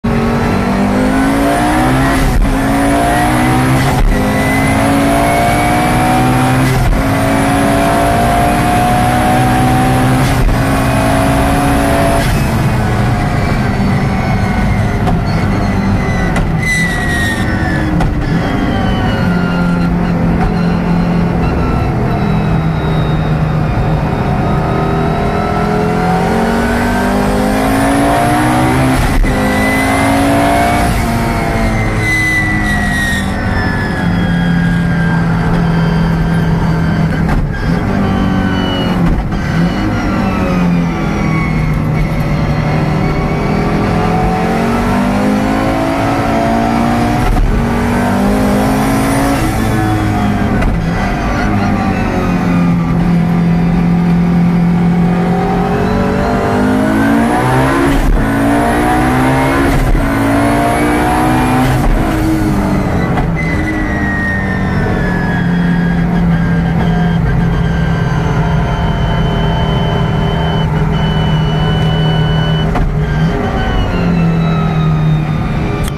Marius Solheim in his bad-ass Audi S2!
This is Marius' first track-day with the big turbo and new-fitted 6-speed dog box. #1